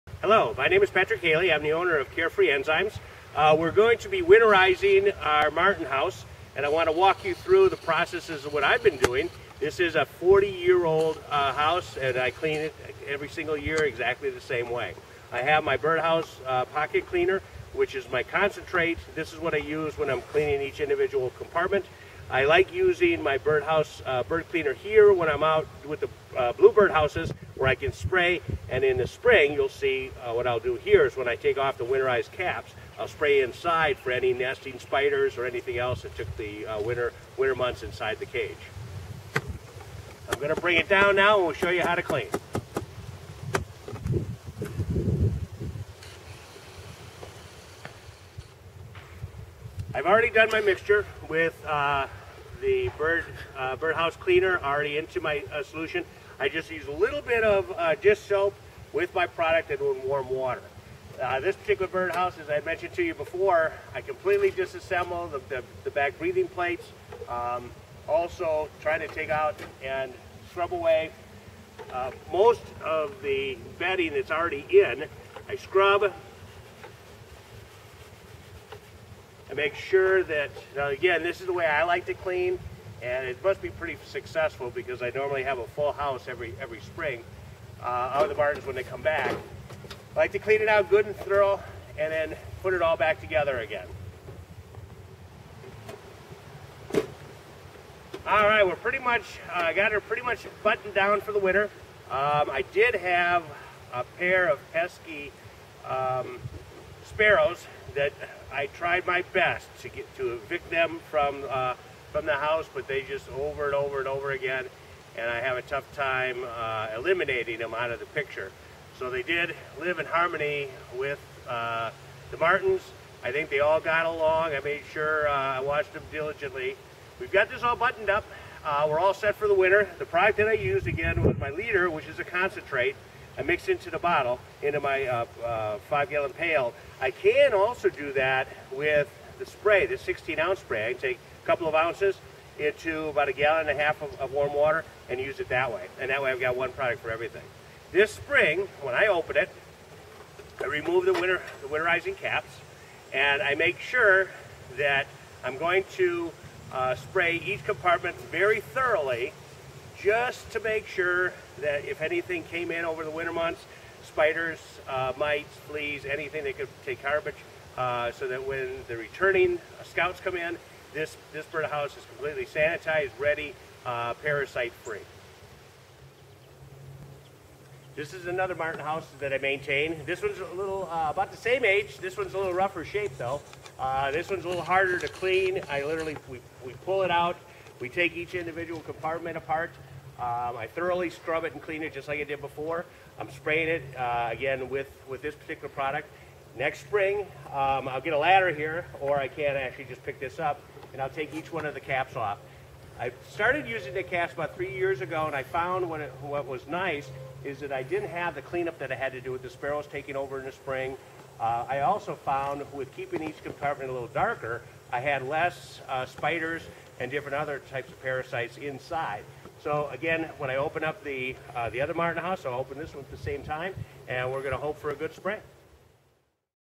Martin House Cleaning Video
Cleaning a Martin House with owner Patrick Haley